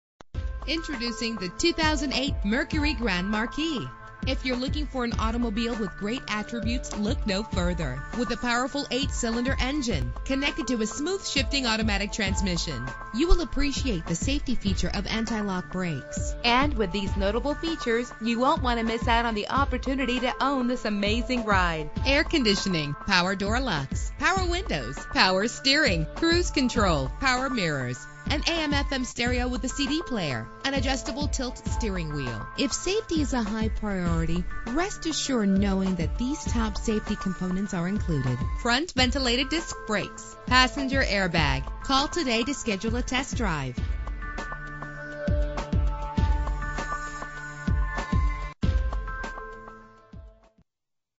2008 Mercury Grand Marquis - Orlando FL
Year: 2008
Make: Mercury
Model: Grand Marquis
Trim: GS
Engine: 4.6 liter 8 cylinder 16 valve
Transmission: 4-Speed Automatic
Color: Blue
Mileage: 27809
Address:
9001 E Colonial Dr